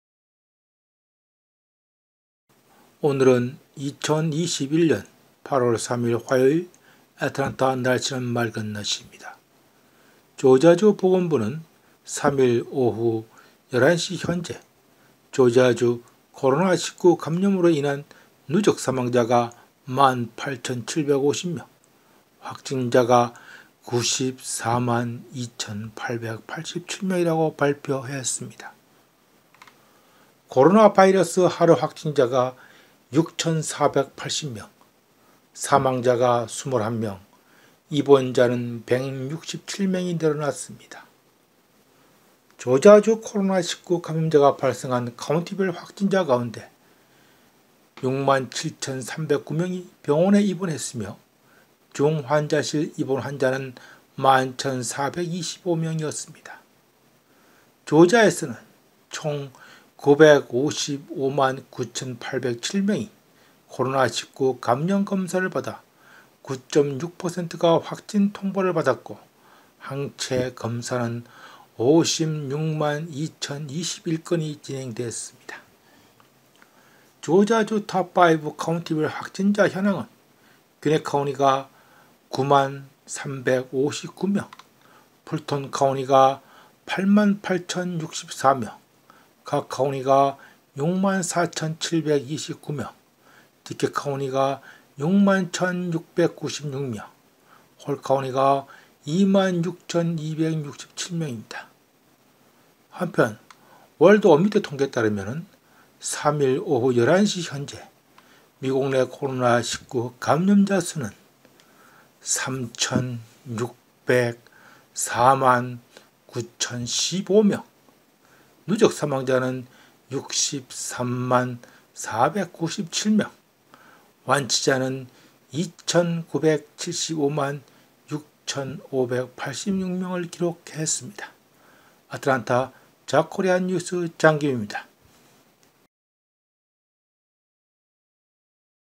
조지아주 코로나19 하루 확진자 6,480명 입원자 167명
조지아주,코로나19,확진자,애틀랜타 뉴스
조지아주 보건부는 3일 오후 11시 조지아주 코로나19 감염으로 인한 누적 사망자가 18,750명, 누적확진자가 942,887명이라고 발표했다.